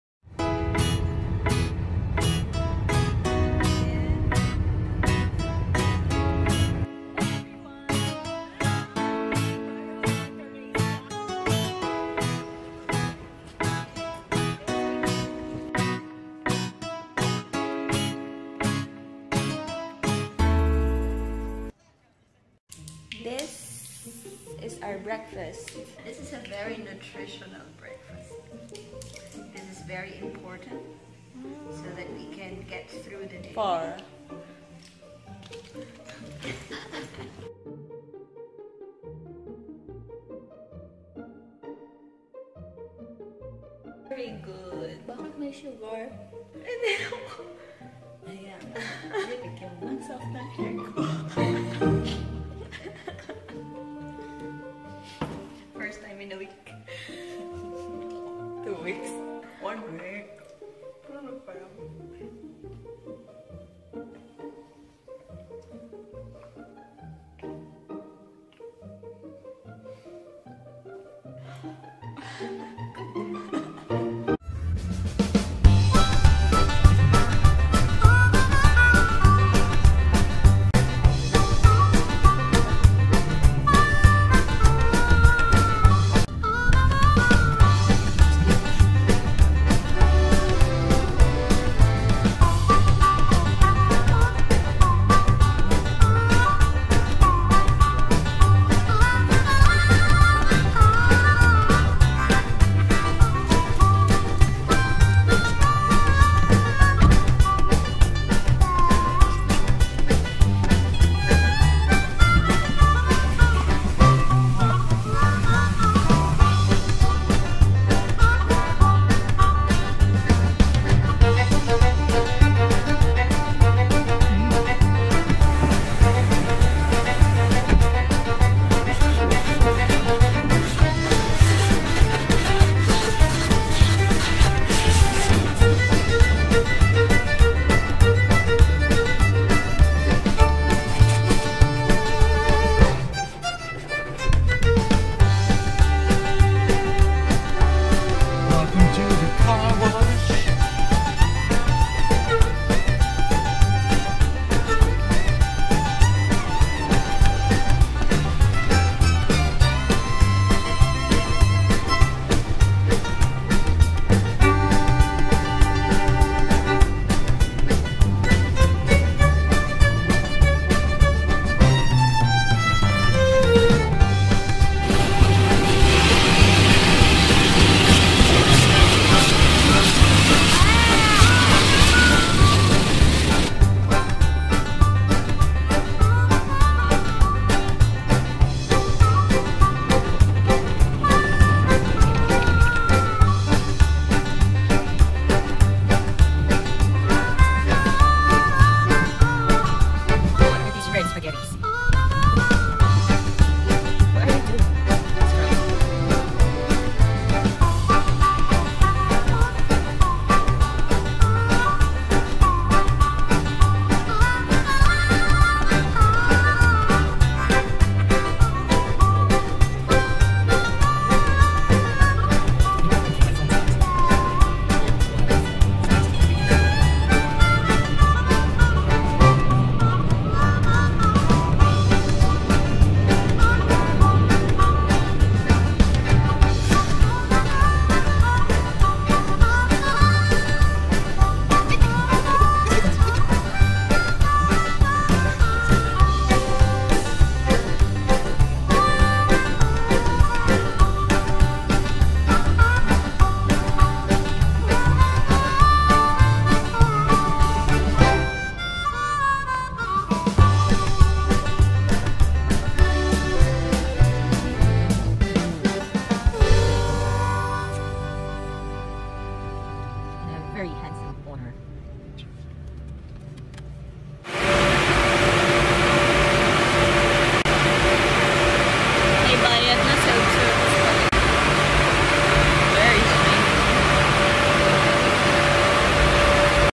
LET'S GO TO THE CAR WASH, THE NETHERLANDS | Dutch & Filipina | Netherlands & Philippines
We went to the car wash and had my boyfriend's car cleaned up. It was my first time experiencing that.

Enjoy watching! :D